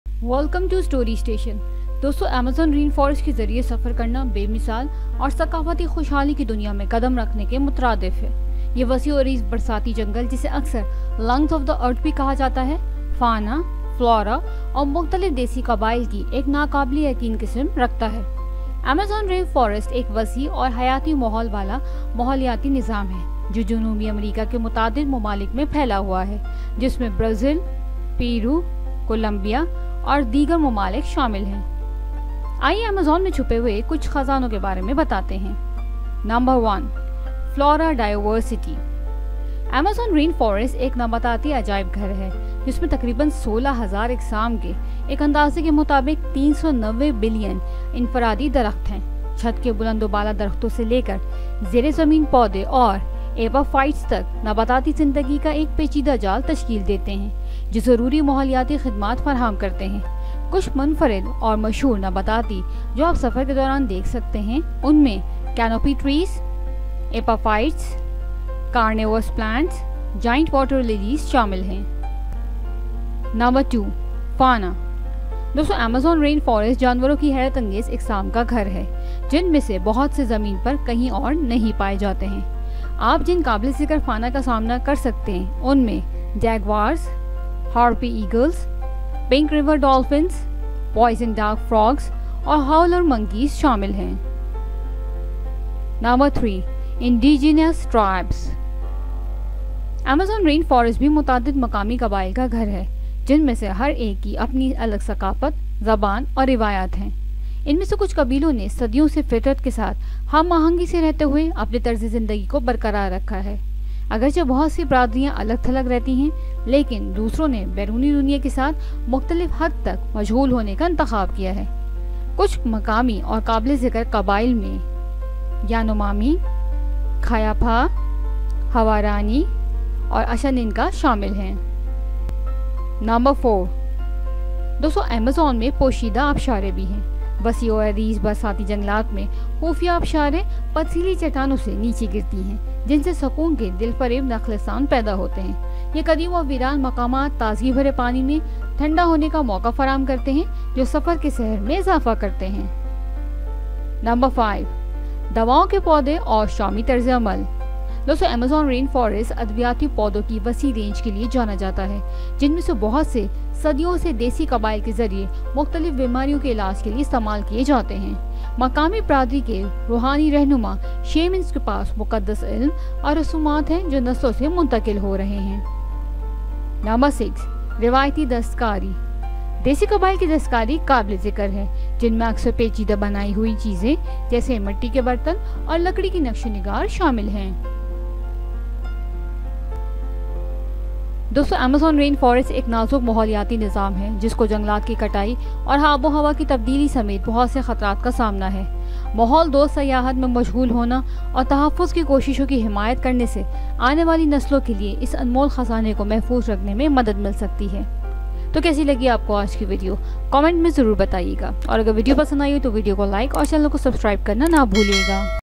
Exploring the Hidden Treasures of the Amazon Rainforest | Story Station
What’s Hidden Behind 2, 124,000 Square Miles of the Unexplored Amazon Forest?

Join us on an awe-inspiring adventure as we venture into the heart of the Amazon Rainforest, unveiling its unique flora, fascinating fauna diversity, and vibrant indigenous cultures. From the mystical shamanic practices to breathtaking encounters with elusive wildlife, this expedition promises to be a journey of a lifetime. Embrace the spirit of ecotourism and conservation as we explore the Tropical Paradise, preserving its wonders for generations to come. 🐾🌳